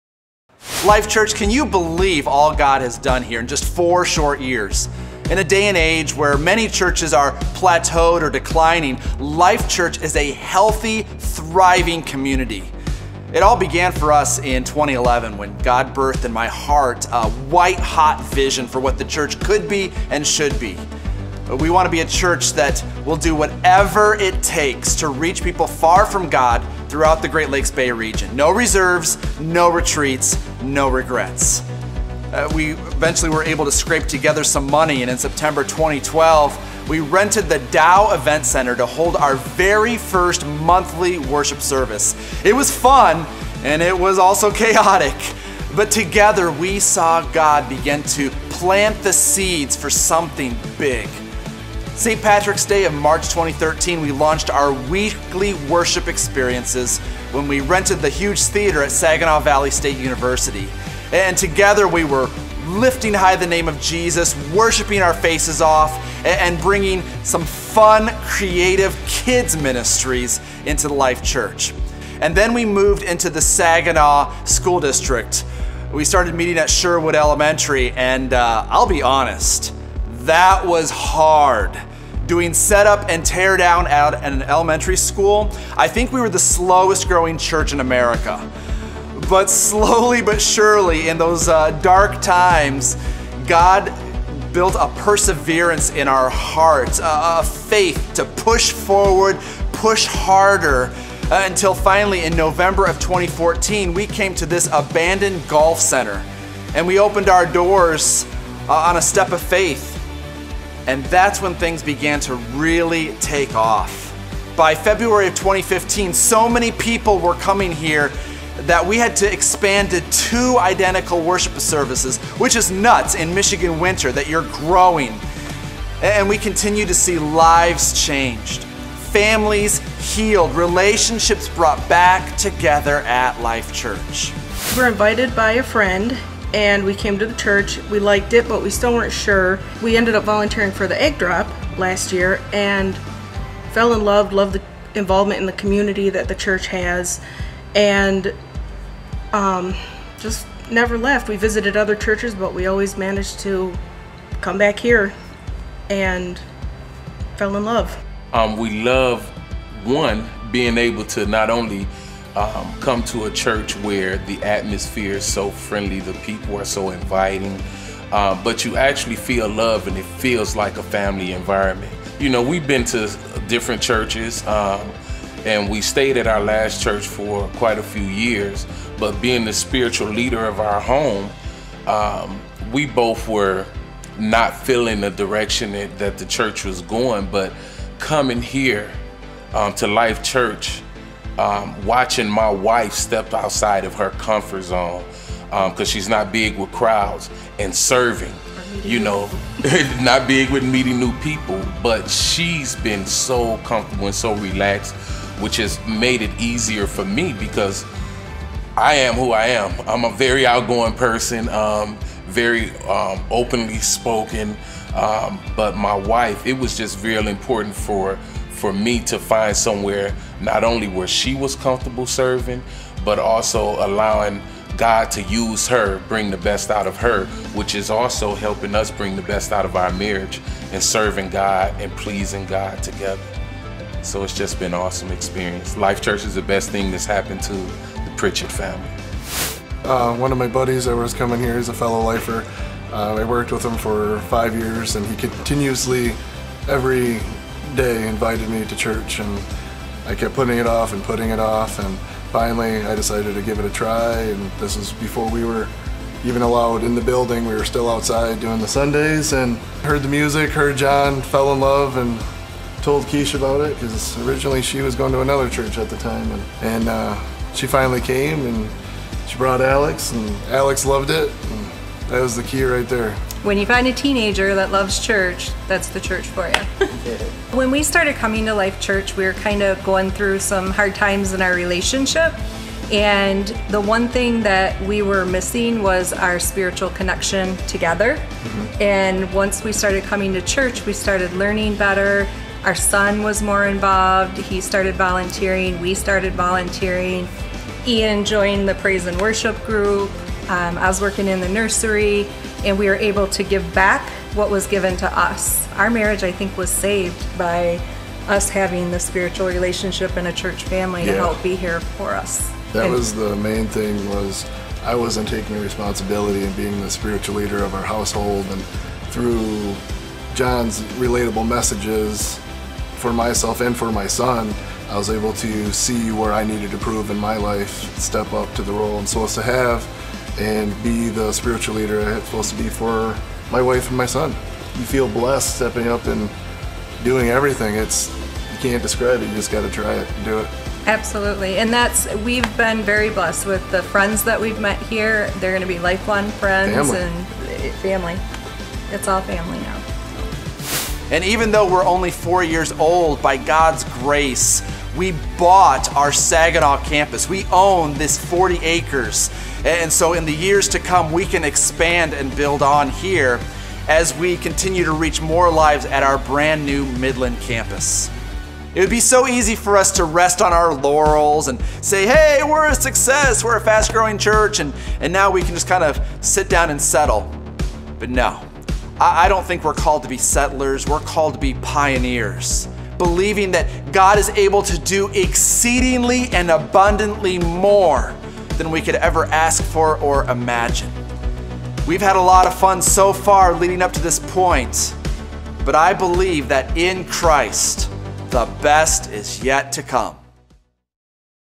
A Brief History of Our Brief History
Life Church Michigan is a fast-growing, 4-year-old start-up that is seeing hundreds and hundreds of lives changed by God.  Here is how we got to here!  LifeChurchMichigan.com